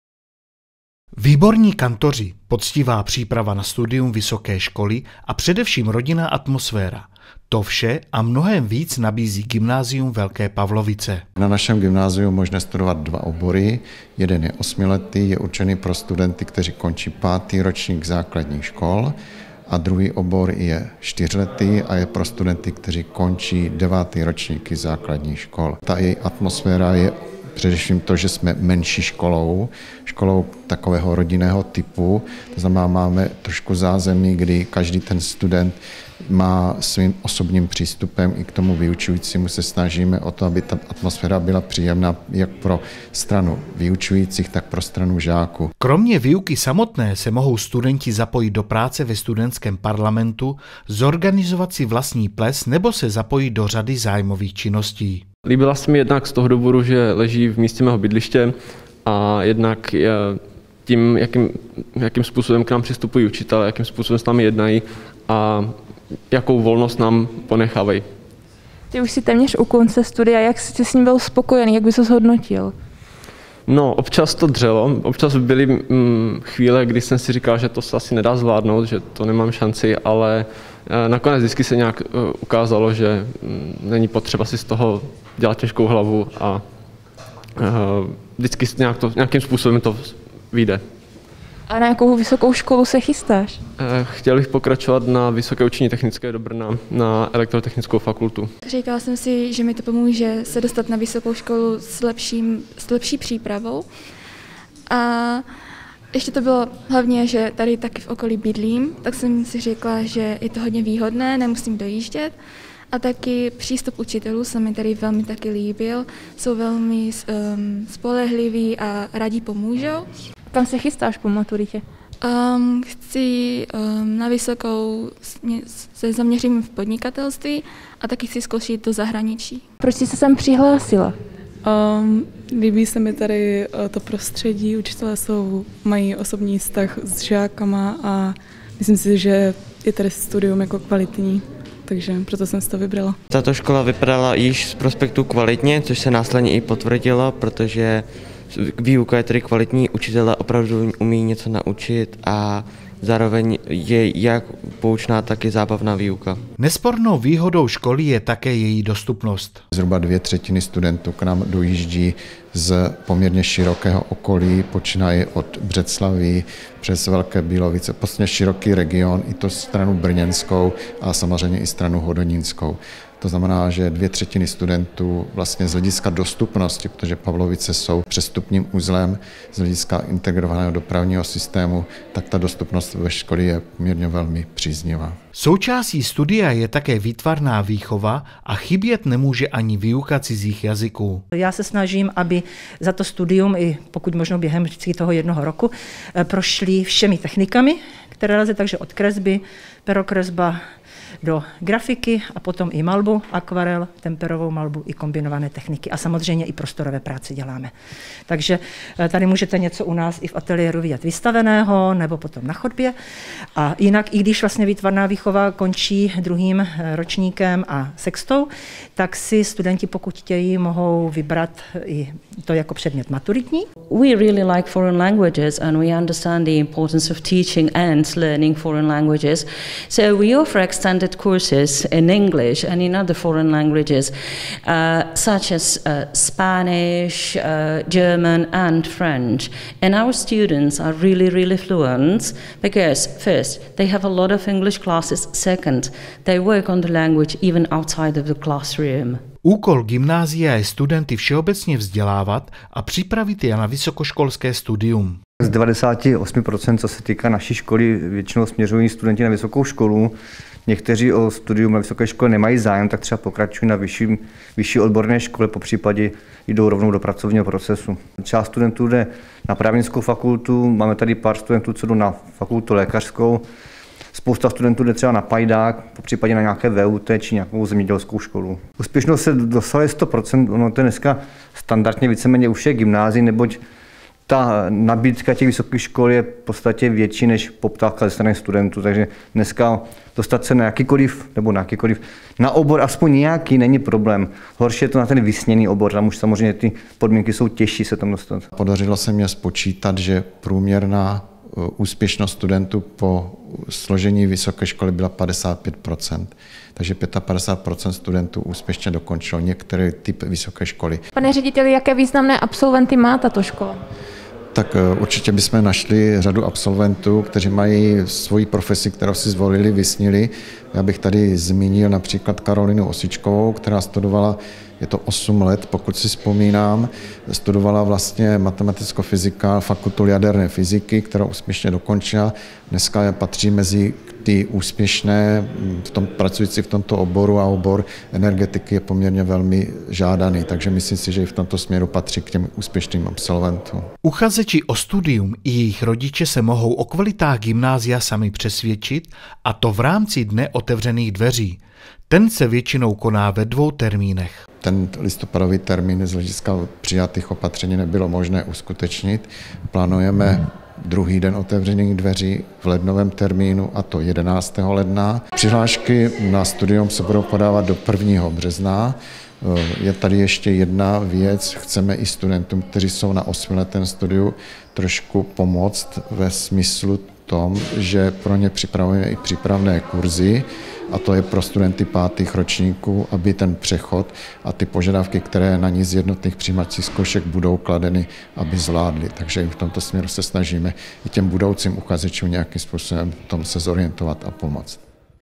Co nabízí studium velkopavlovického gymnázia?
Výborní kantoři, poctivá příprava na studium vysoké školy, a především rodinná atmosféra – to vše a mnohem víc nabízí Gymnázium Velké Pavlovice.